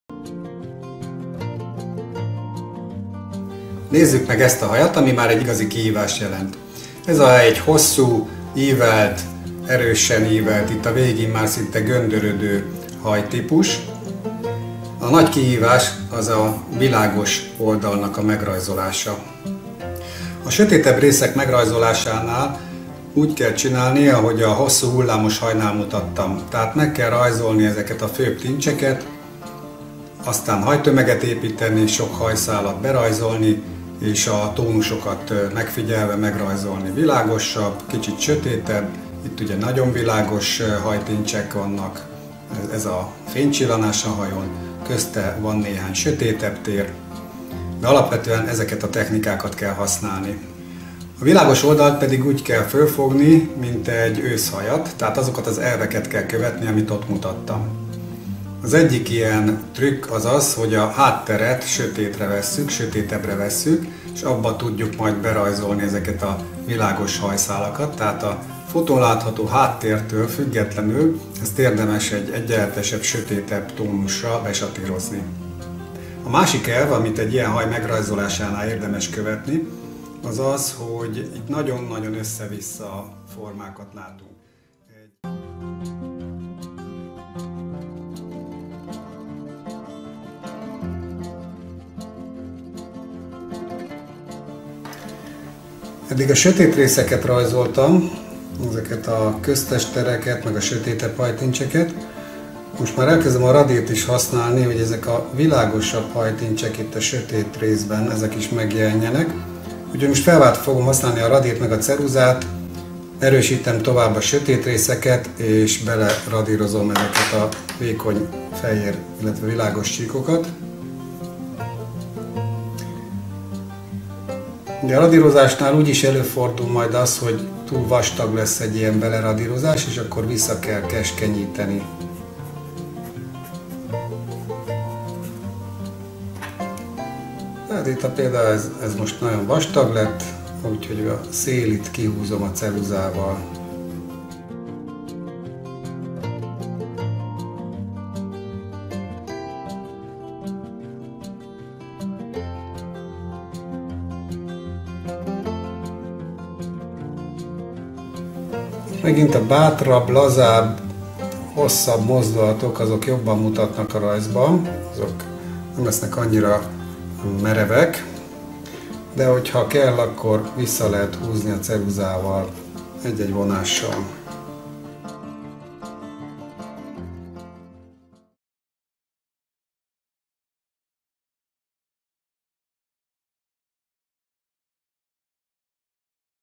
Rajz Rajongók Klubja Videómagazin: A hajrajzolás alfája és omegája - göndör haj
Egy portré sosem lehet tökéletes, ha nem elég részletes. Az egyik legfontosabb eleme a haj ábrázolása, ebbe sok embernek beletörik a bicskája. Kukkants bele a göndör haj rajzolásának lépéseibe.

 @Valdor Art - Jobb agyféltekés rajzolás ​